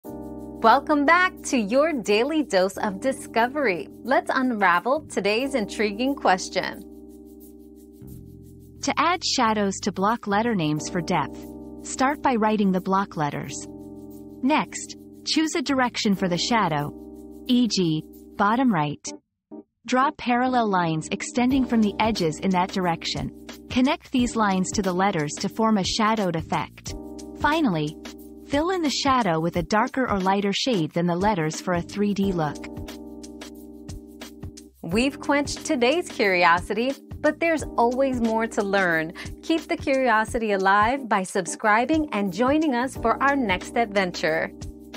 How do you add shadows to block letter names for depth?
Creating Depth: Adding Shadows to Block Letter Names 👉 Depth Shadows Tutorial 👉 Learn how to add shadows to block letter names for a 3D effect with this easy step-by-step tutorial.

Laura S. Harris (2024, May 23.) How do you add shadows to block letter names for depth?
    🌐 AskAbout.video